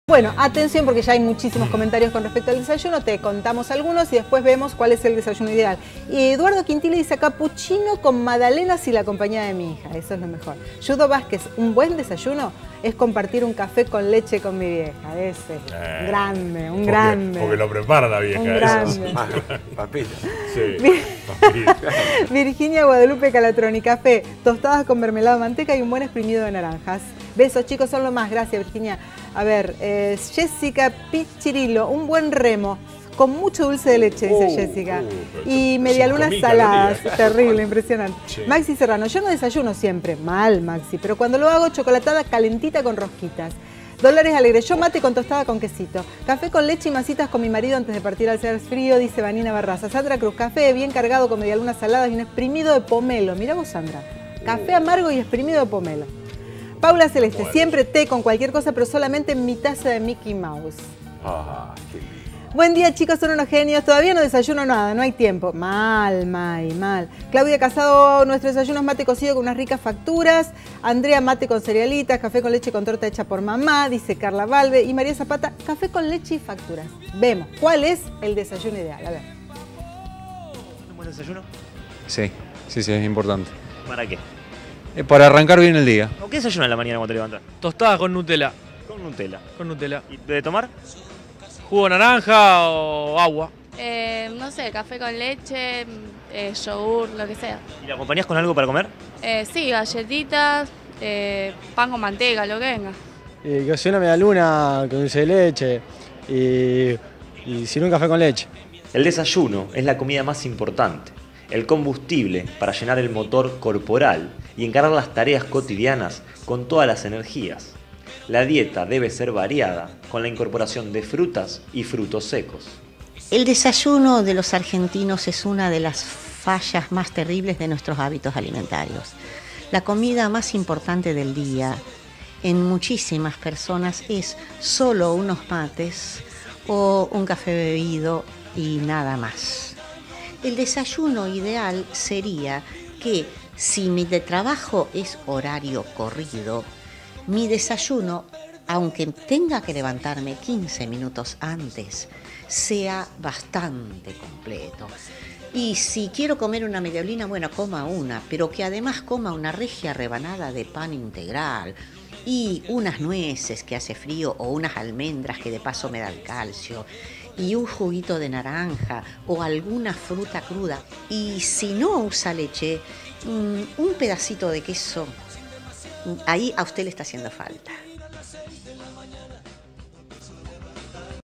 CANAL 5 -- ROSARIO -- BIEN TEMPRANO. ¿CUÁL ES TU DESAYUNO IDEAL?
Qué comer a la primera hora del día // CRISTINA DE LA MATA, médica endocrinóloga -- mat. 6562